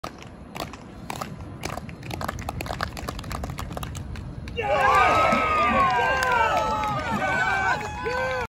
Shelby McEwen - High Jump - 2.30m (7'6.50") - 2024 USATF Regional Championship - June 15, 2024
USATF Regional Championship - Rice University

2.16 o
2.21 o
2.30 xo
2.34 xxx